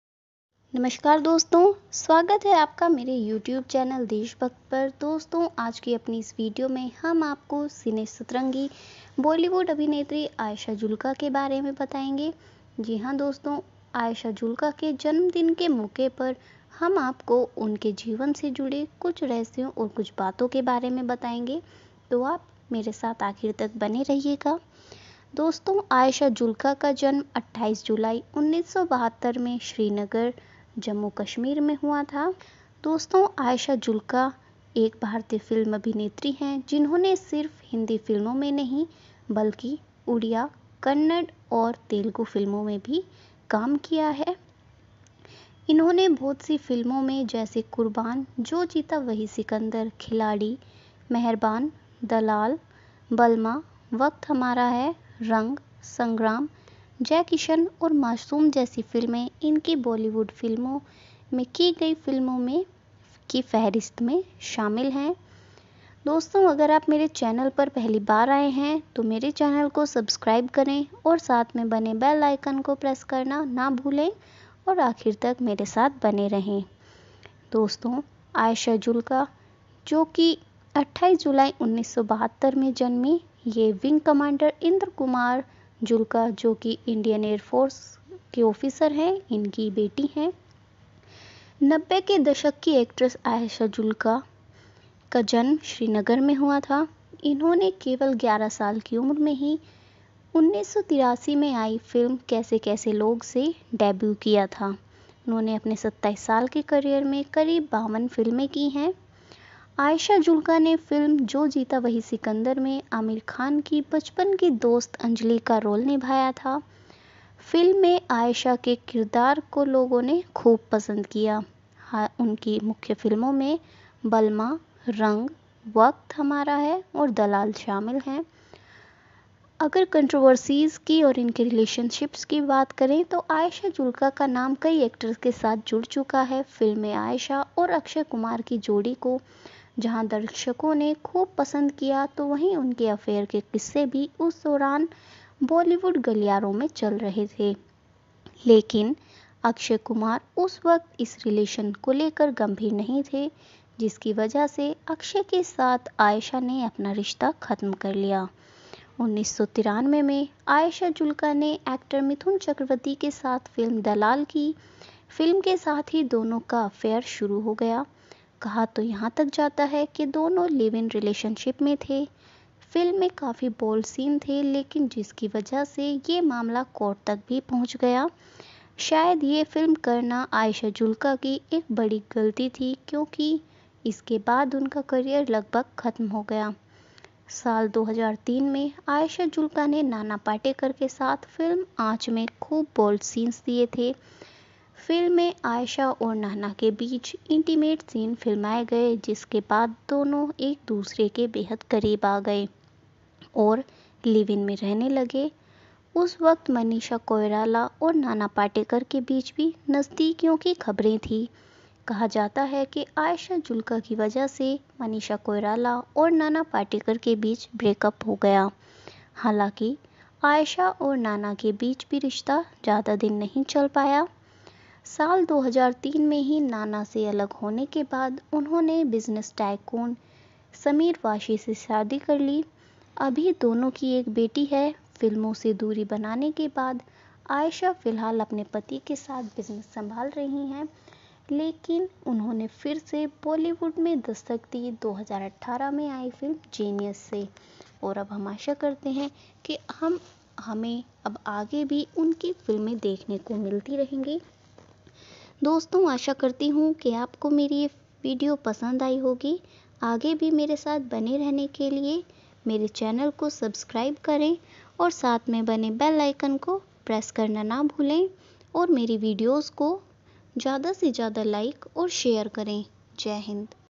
आयशा जुल्का, (ayesha jhulka) एक चुलबुली हेरोइन की हसीन कहानी।
देशभक्त/deshbhakt चैनल पर आज आपको खूबसूरत हीरोइन आयशा जुल्का के जीवन ,फैमिली बैकग्राउंड,फिल्मी केरियर, लव लाइफ के बारे में बताया जा रहा है।